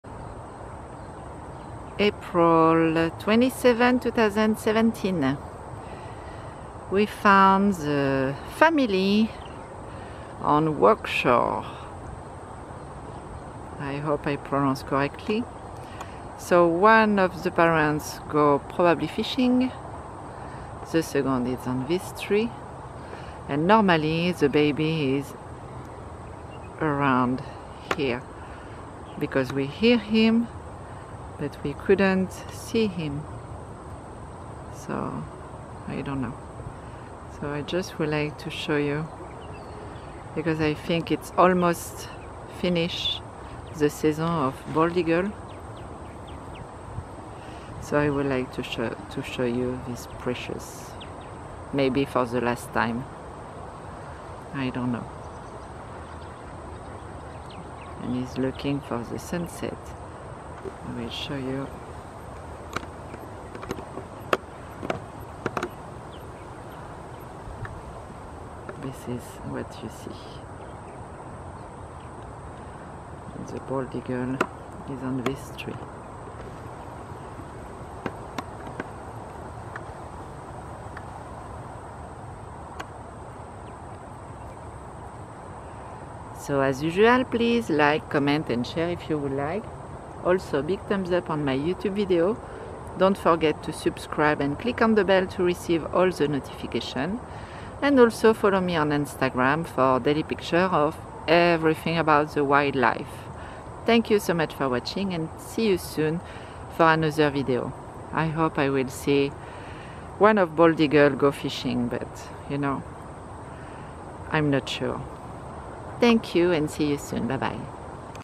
The Woodlands TX Bald Eagles - Martha & George W. - 04.27.2017
Finally after two day of research, I have found the couple in the island on the Lake' Woodlands in the evening. I guess it will be my last video of the season of this majestic bird. They will migrate till the end of the year.

 Enjoy this last movie and feel free to look at all the videos during the next 6 months until they will be back at their nest (I hope) Start at the last page 7 and come back to the page 1. If you click on see all it will be as a big movie of them. Enjoy and see you soon